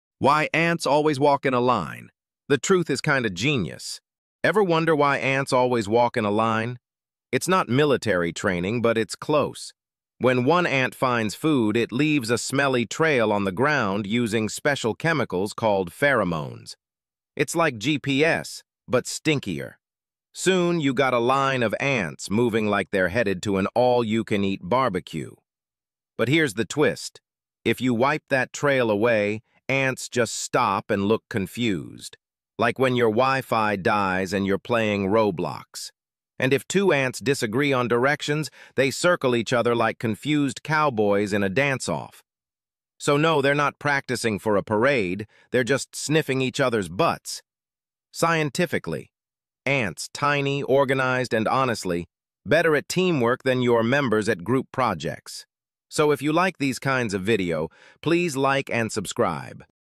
Shocking Ant Secret: Why They Always WALK In LINE🐜👀
Ants Don't Follow Orders — They follow butt-stinks thinking its a gps. How it really works...

why ants walk in a line
ants in a line explained
weird animal behavior
ant pheromone trail
true insect facts